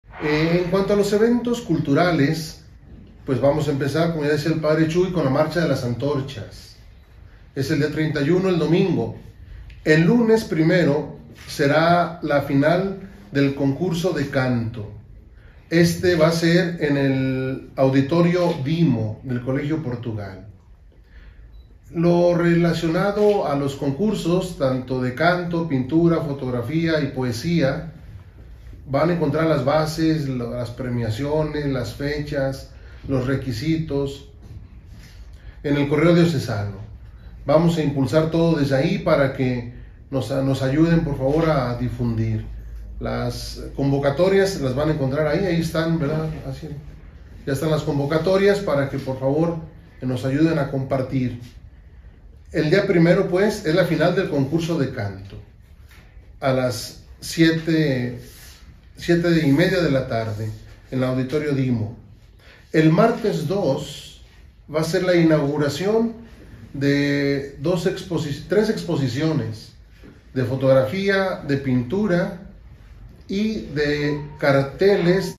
Eventos culturales irán de la mano con el Quincenario en honor de nuestra Señora de la Asunción
A la par de las peregrinaciones y la Romería en honor de nuestra Señora de la Asunción durante el Quincenario se llevará a cabo un amplio programa de eventos culturales, de acceso libre para todos los habitantes de la Diócesis de Aguascalientes y visitantes que se encuentren interesados.
De acuerdo a lo dado a conocer por le presbítero Jaime Silva Castañeda, el 1 de agosto de realizará la final de Concurso de Canto, en el Auditorio Dimo; el 2 de agosto, en la Casa Terán tendrá verificativo la Inauguración de la Exposición de fotografía y pintura; el día 4 de agosto, la conferencia a 25 años de la promulgación del Catecismo de la Iglesia Católica, a las 20:30 hrs en Catedral; también en Catedral¡, el 5 de agosto se tiene programado el panel mariano “Con María, en comunión y participación. Una visión Bíblica, teológica y pastoral”.
También a las 20:30 horas en Catedral, pero el 6 de agosto se realizará la presentación del libro “Las fiestas patronales en Aguascalientes: 8o años del Quincenario y 60 años de la Romería de la Virgen de la Asunción”.
Posteriormente, el 8 de agosto en la Casa Terán tendrá verificativo la final del concurso de pintura; ese mismo día a las 20:30 en Catedral se presenta el mariachi Imperial Azteca y las Voces de Aguascalientes; el 9 de agosto en el Teatro Morelos se realizará el Ferial del Quincenario de la Asunción y el 10 de agosto en la Catedral, la mesa redonda “Históricos del Quincenario y la Romería”.
A las 20:30 horas del 11 de agosto en Catedral se presentará la Banda Municipal, en tanto que a la misma hora del día 12, también en Catedral ofrecerá un concierto la Orquesta Filarmónica de Aguascalientes.
Por último dentro del programa Cultural del Quincenario en honor de Nuestra Señora de la Asunción, el 13 de agosto a las 20:30 en Catedral se llevarán a cabo las conferencias marianas “Maravillas hizo en mí y las puede hacer en ti” y el 14 de agosto a las 21:00 horas, también en Catedral se ha programado la actuación de Coro Diocesano.
Todos los eventos son gratuitos y no requieren boleto.